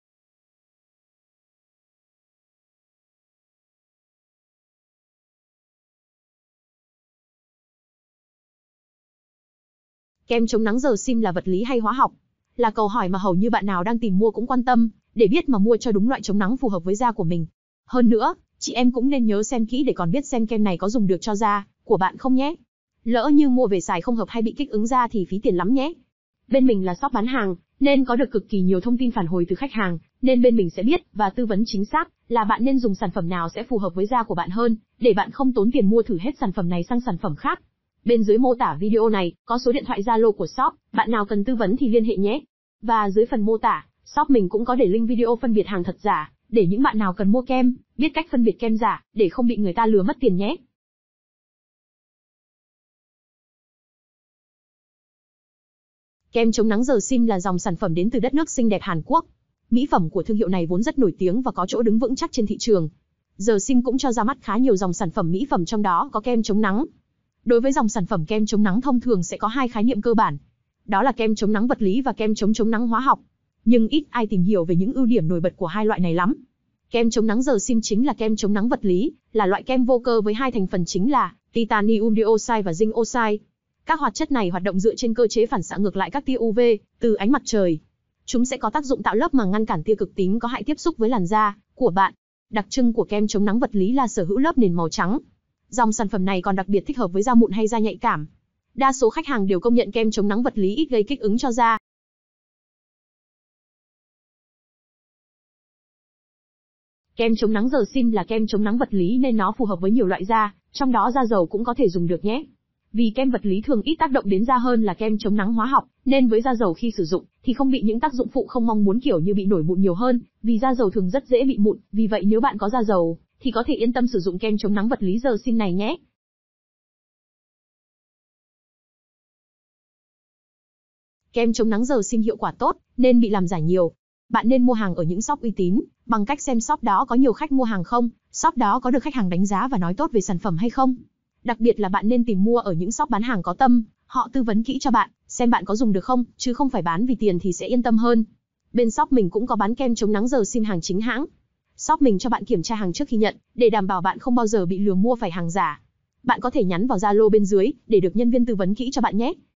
Kem Chống Nắng The Saem Hồng Là Vật Lý Hay Hóa Học Dùng Cho Da Dầu Được Không?
Đối với dòng sản phẩm kem chống nắng thông thường sẽ có hai khái niệm cơ bản. Đó là kem chống nắng vật lý và kem chống chống nắng hóa học. Nhưng ít ai tìm hiểu về những ưu điểm nổi bật của 2 loại này lắm. Kem chống nắng giờ Sim chính là kem chống nắng vật lý, là loại kem vô cơ với 2 thành phần chính là: Zinc Oxide hoặc Titanium Dioxide.
➡ Xem video cách phân biệt kem chống nắng The Saem thật giả ở đây: